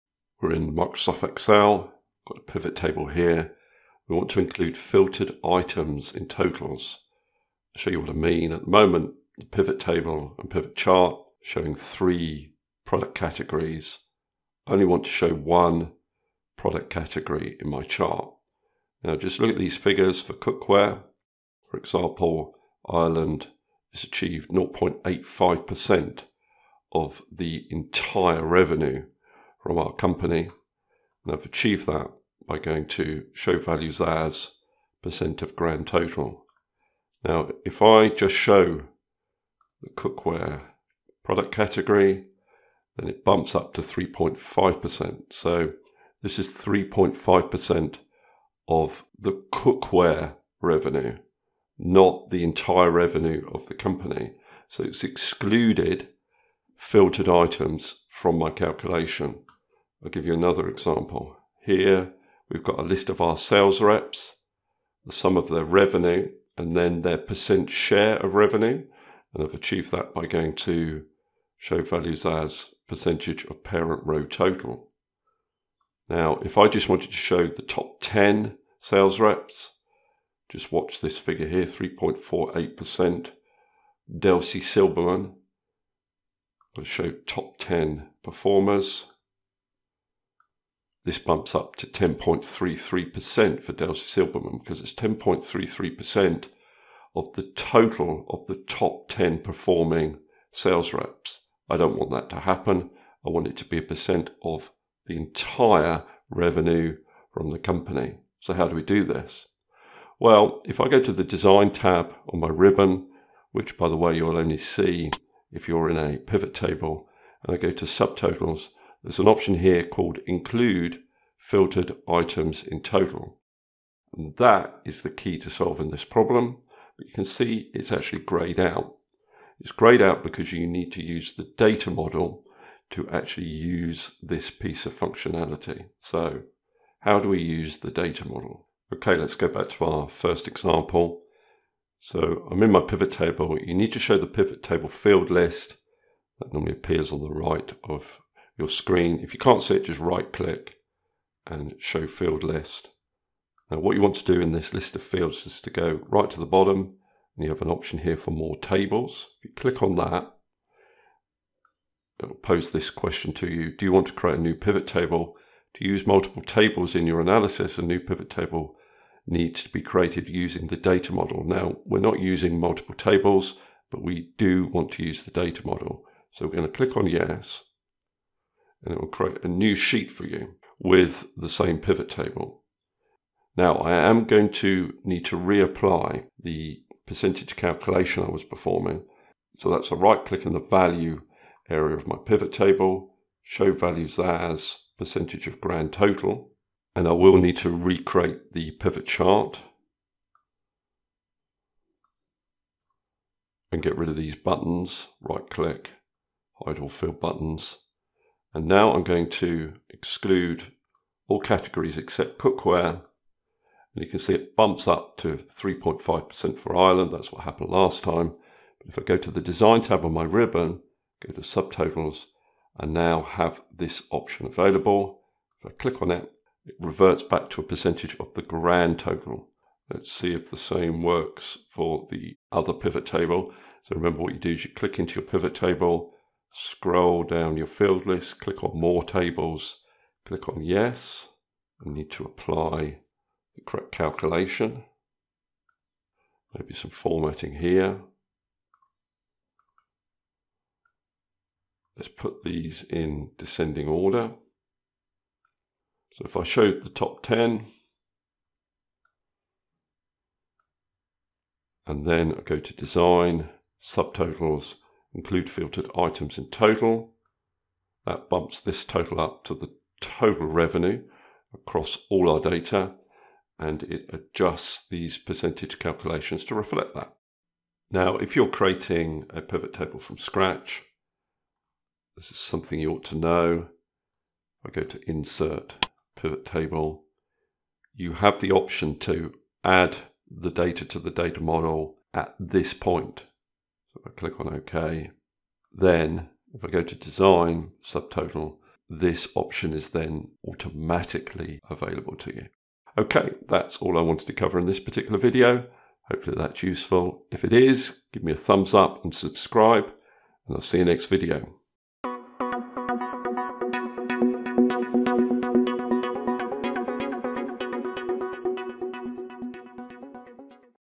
PivotTables: Filter Without Changing Total | Display Percentage of Total When Filtered
In this video I am working with PivotTables in Microsoft Excel.  I want to be able to filter columns or rows without changing the grand-total calculations in the PivotTable: this is especially relevant to percentage calculations. By default percentage calculations will be based on the total of visible rows or columns and exclude filtered items.

There is an option in the Subtotals menu that allows you to 'Include Filtered Items in Totals'.  However, unless you have added your data to the data model this option is greyed out.  This video will demonstrate how to add existing PivotTables to the data model and therefore enable this option.

Table of Contents:

00:00 - Introduction
00:07 - Problem 1: Percentage calculations with filtered PivotTable columns
00:59 - Problem 2: Percentage calculations with filtered PivotTable rows ( eg Top 10 )
01:49 - "Include filtered items in totals" greyed out
02:21 - Add an existing PivotTable to the data model
04:37 - Add data to the data model when creating a PivotTable from scratch